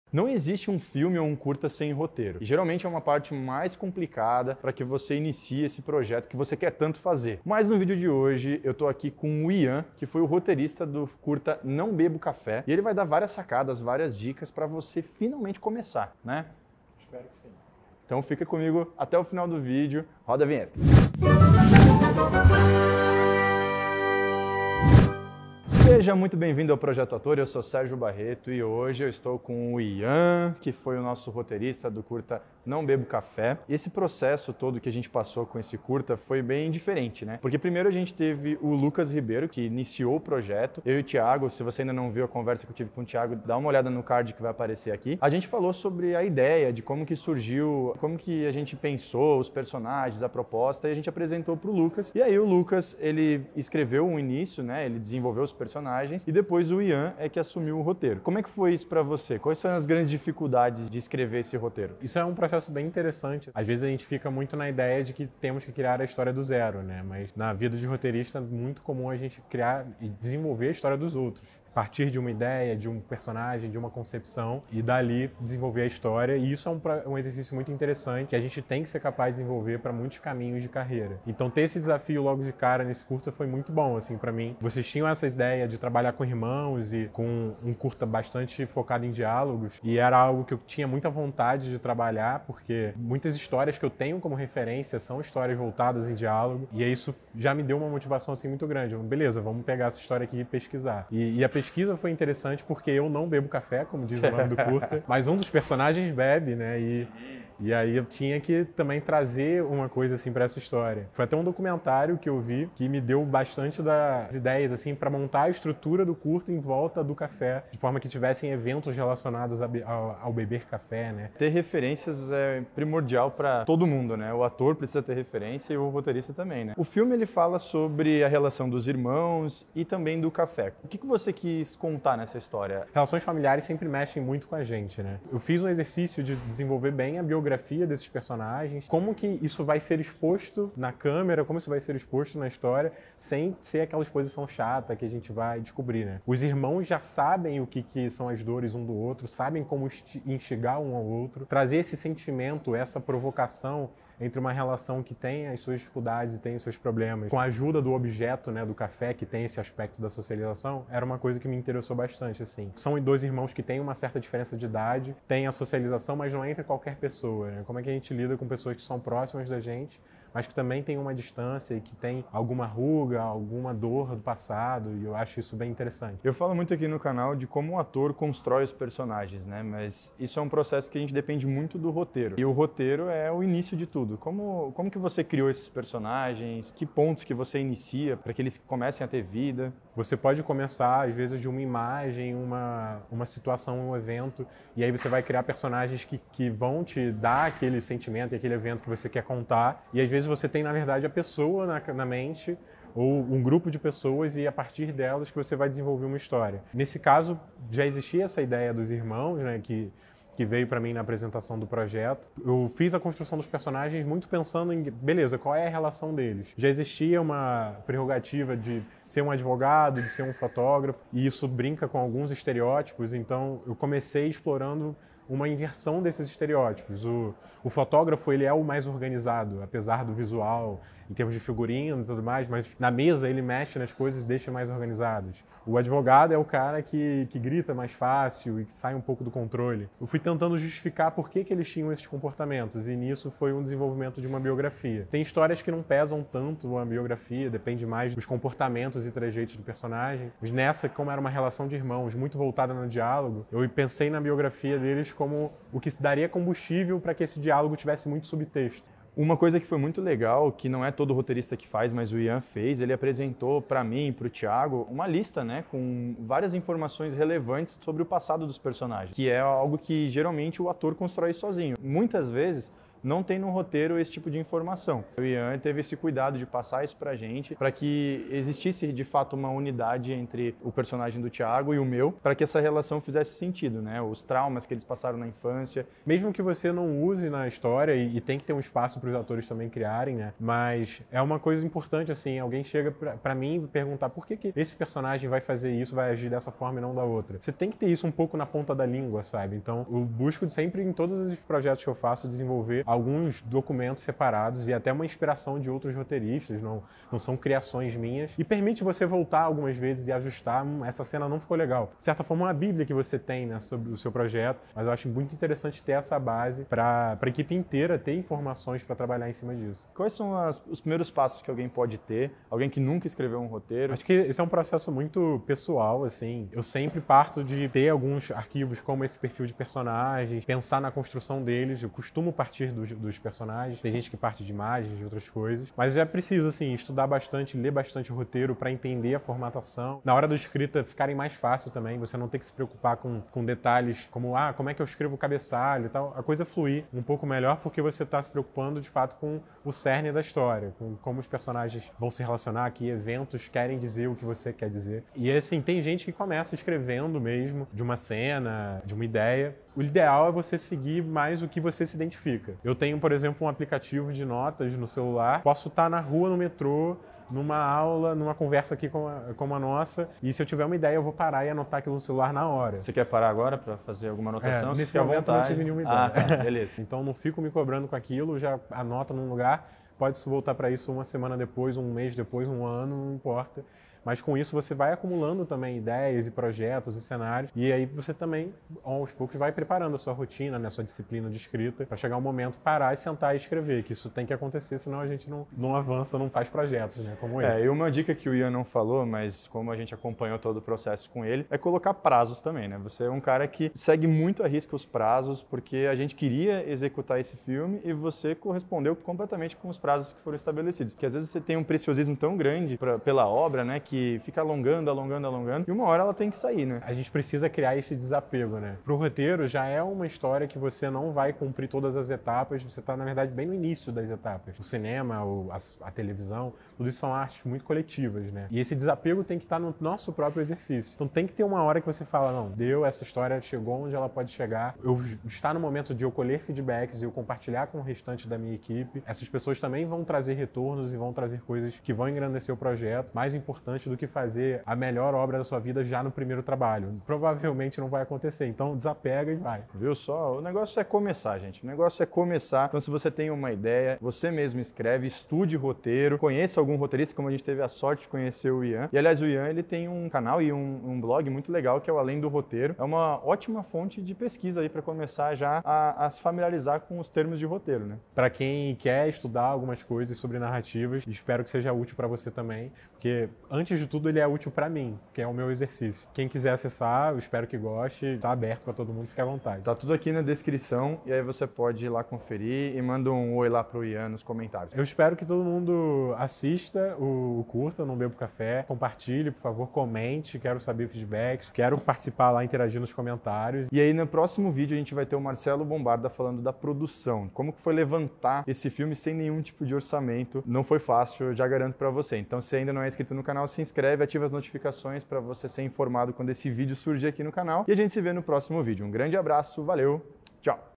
COMO ESCREVER UM ROTEIRO DE CURTA METRAGEM? (com Yann Rodriges - Além do Roteiro) | Projeto Ator 171
Meu CURSO ONLINE para você que quer dar os primeiros passos na atuação:
*PROJETO ATOR: PRIMEIROS PASSOS

Quer dicas para começar aquele projeto? O roteirista Yann Rodrigues do canal Além do Roteiro, comenta o processo que teve com o curta NÃO BEBO CAFÉ, disponível aqui no canal.

*** FAÇA PARTE DA NOSSA COMUNIDADE - ENTRE NO NOSSO GRUPO DO FACEBOOK